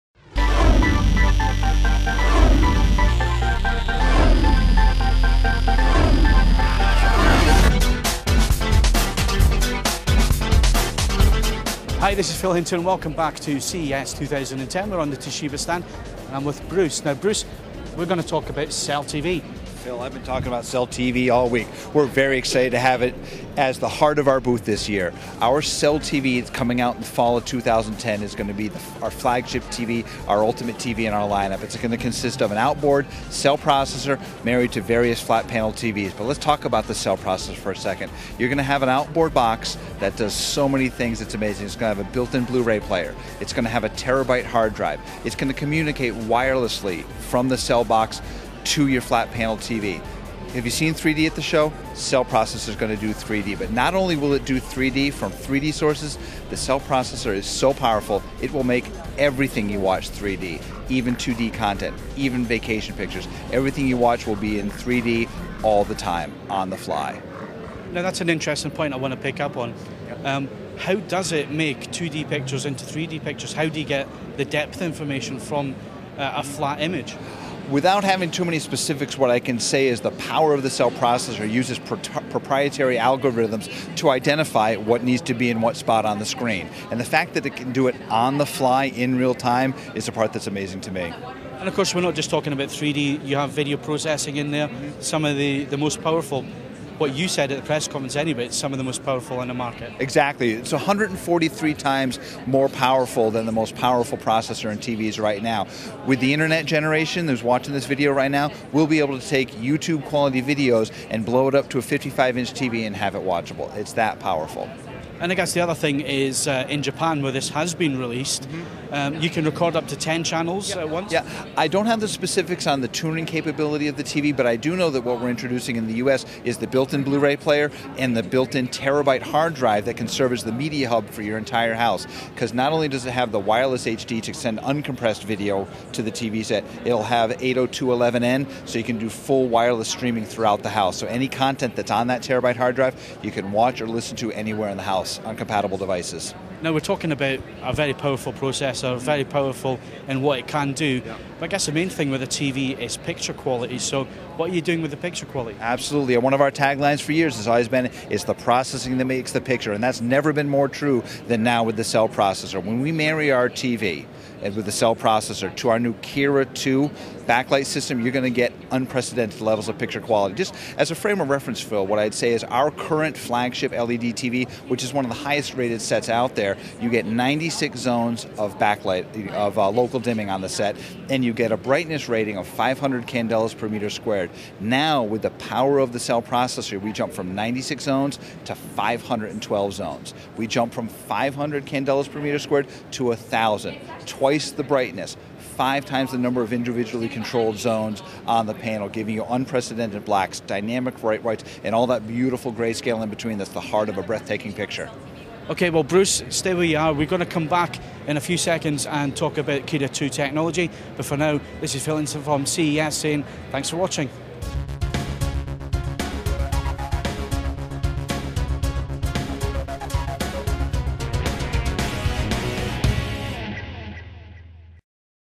CES 2010 - Toshiba CELL TV
Toshiba tell us about their new premium LCD HDTV for 2010. To comment on this video and the technology featured, go